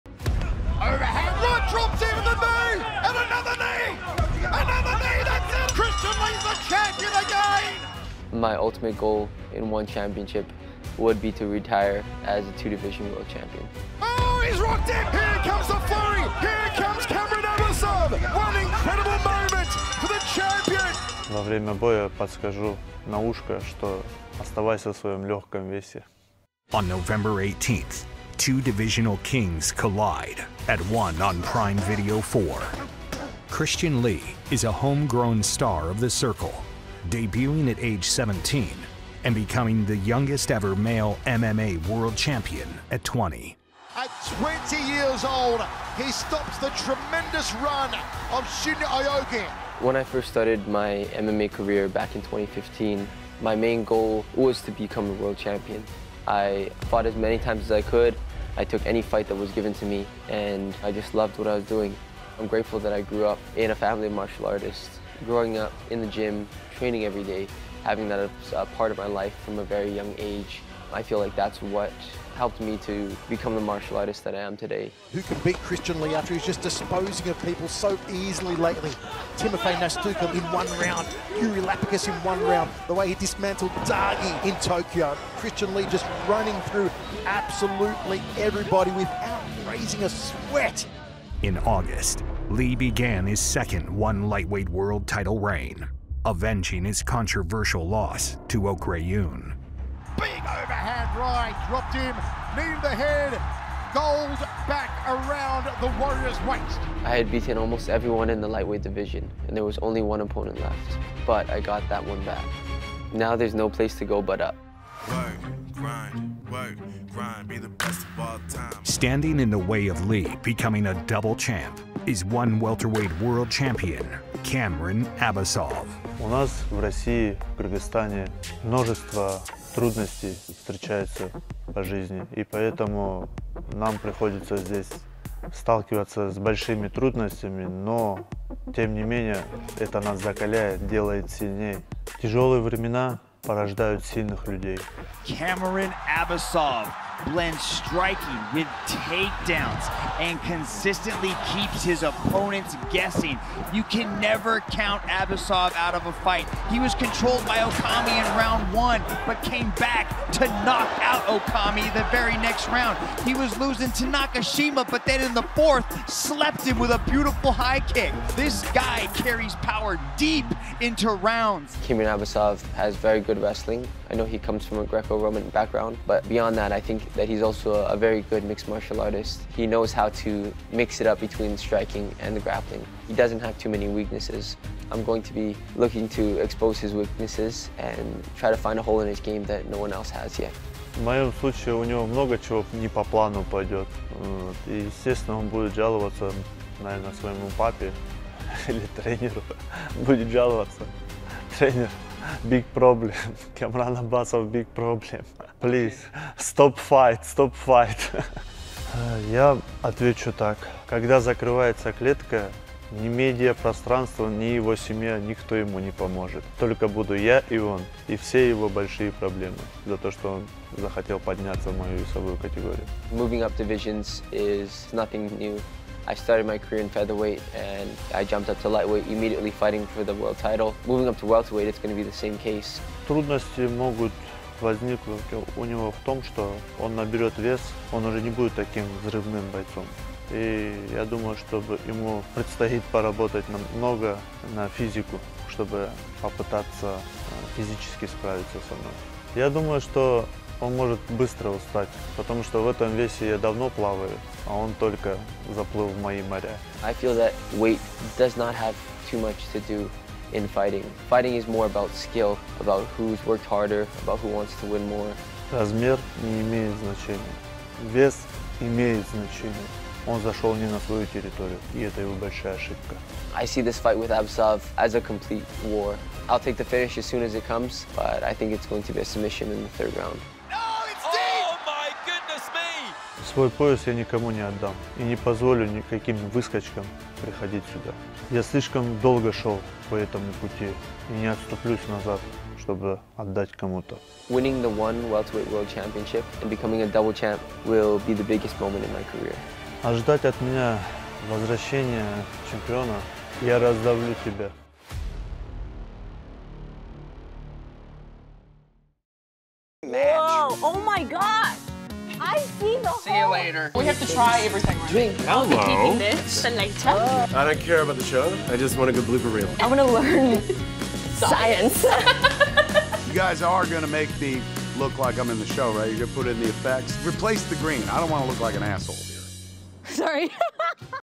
Christian Lee vs Kiamrian Abbasov: Official Fight Preview | Prime Video
Don't miss this FIRECRACKER World Championship 🏆 battle as lightweight king Christian Lee moves up to challenge Kiamrian Abbasov for the ONE Welterweight World Title on November 18 at ONE on Prime Video 4! 🧨